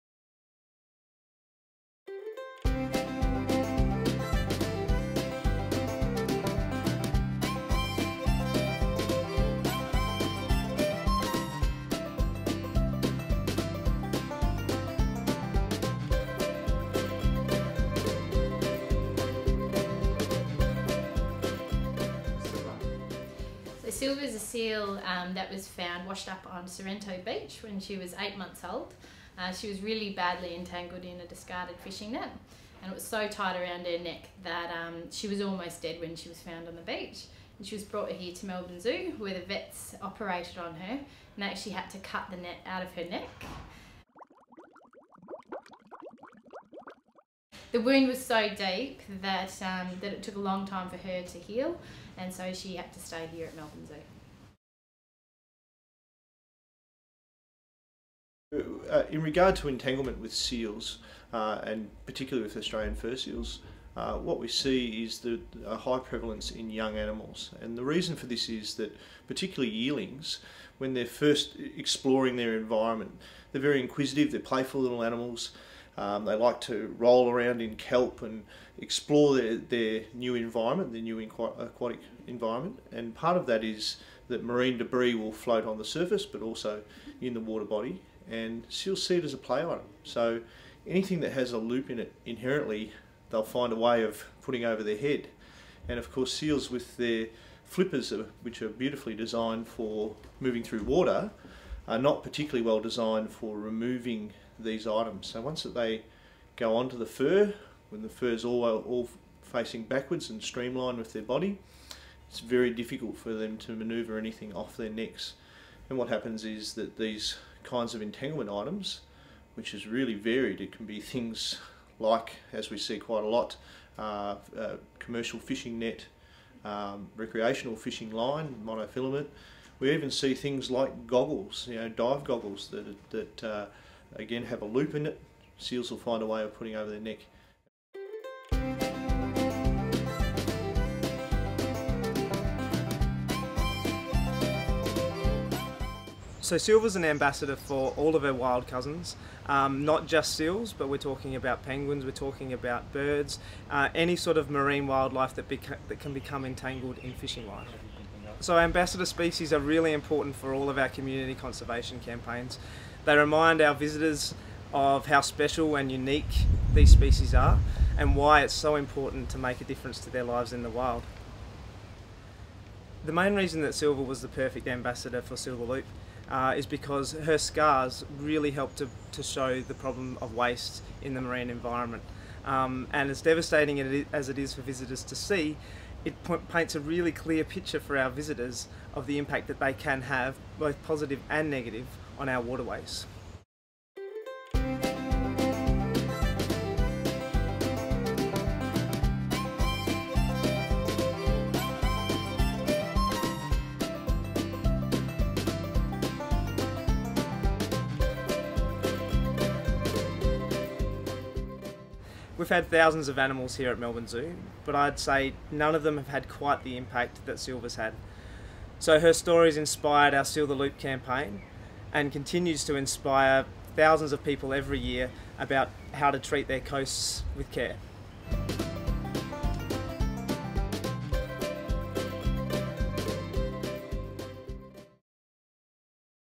Silva the Seal
Watch the story of one of Melbourne Zoo's most famous residents and find out how she has made of of the biggest impacts of any animal at the Zoo!